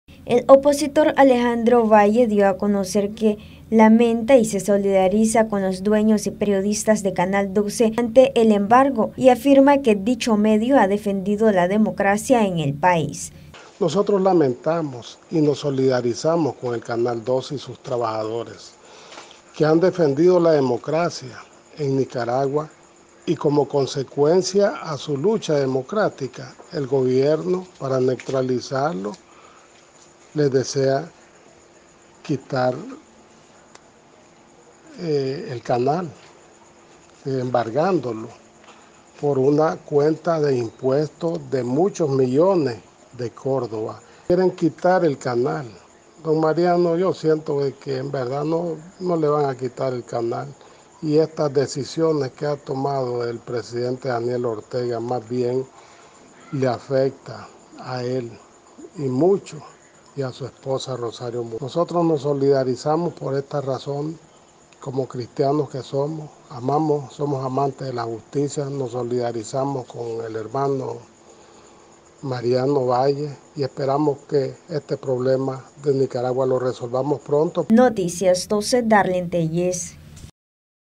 Gobierno debería de reflexionar ante embargo a canal 12
NosResistimosAlEmbargo Embargo a canal 12 se debe que es un medio de comunicación que ha luchado por la democracia junto a los nicaragüenses, dijo el opositor Alejandro Valle.